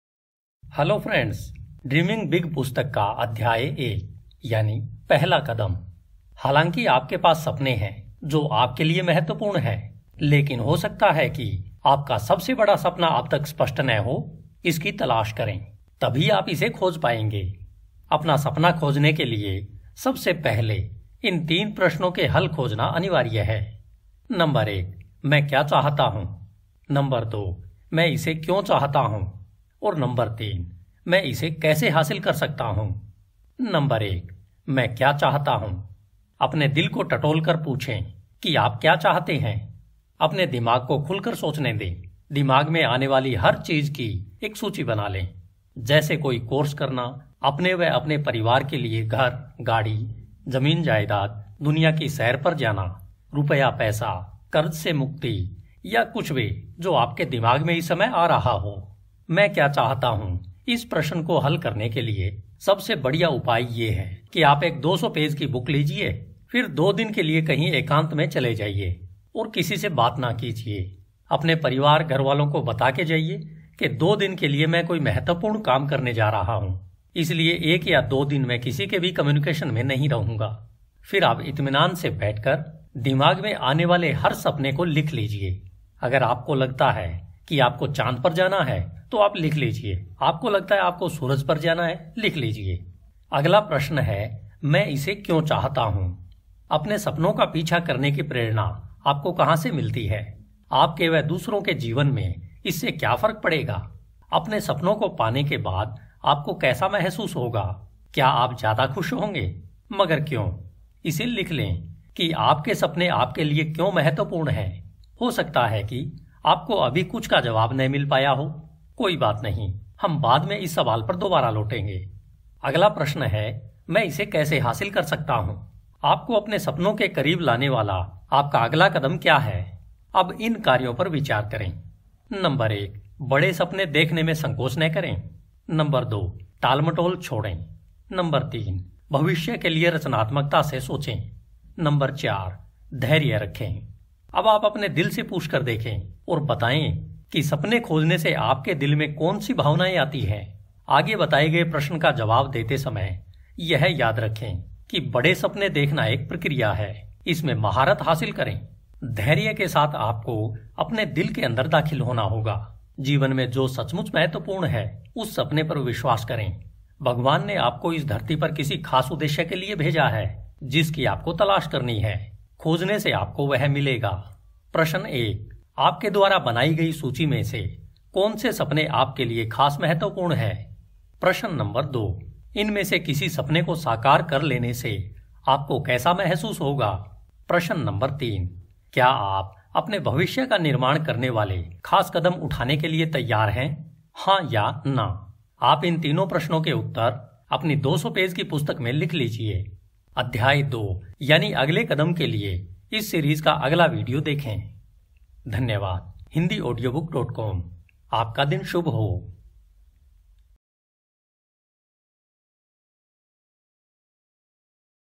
Dreaming Big What? Why? How? audiobook in hindi P1
What? Why? How? a  Dreaming Big audiobook in hindi P1

How to Dreaming Big and connect with your Life Dream. Watch a Video Series on Dreaming Big book in Hindi.